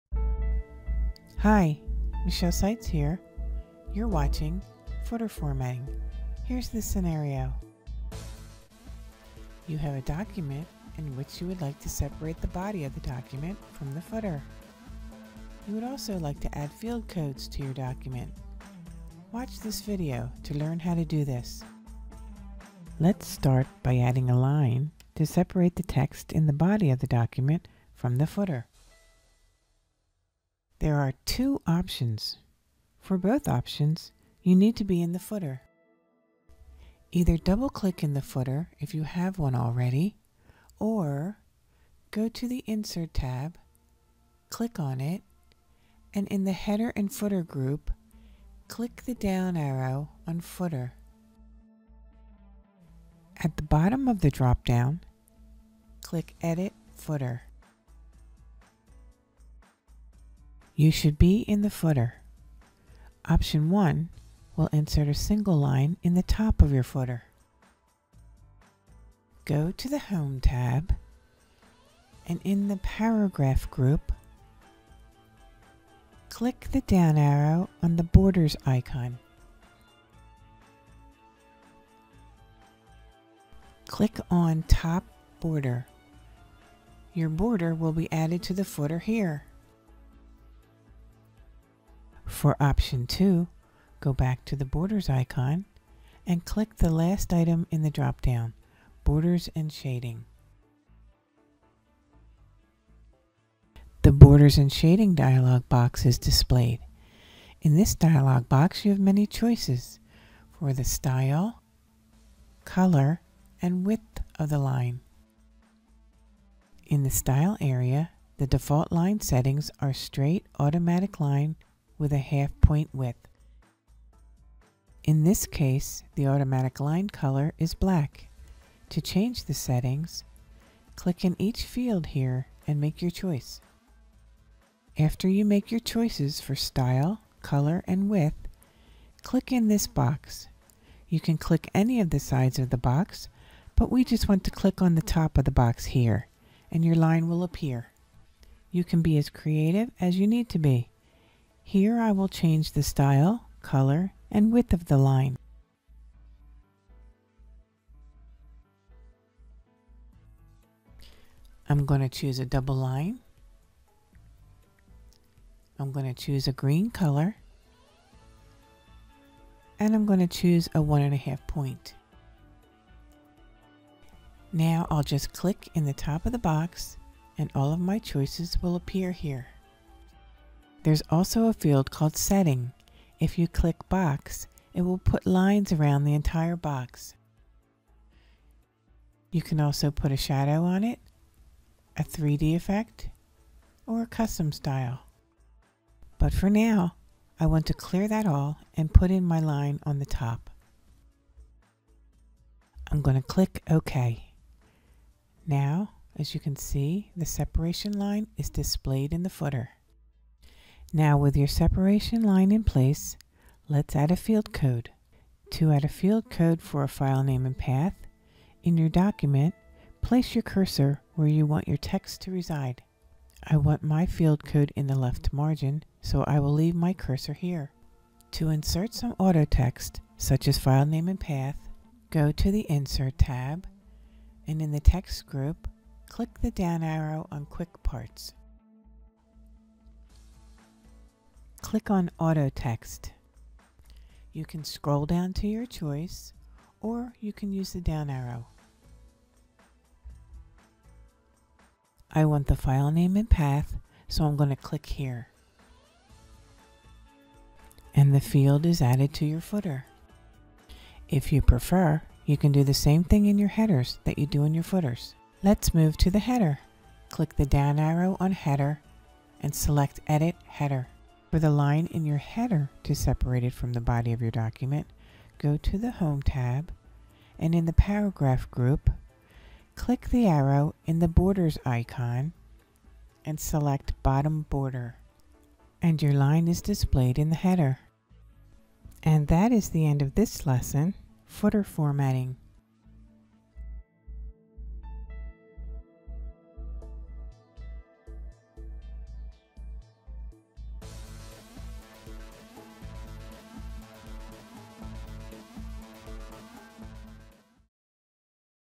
Word 2013 Footer Formatting - Add a separation line & field code
Learn how to insert a separation line in your Footer and/or Header as well as add a field code to your footer.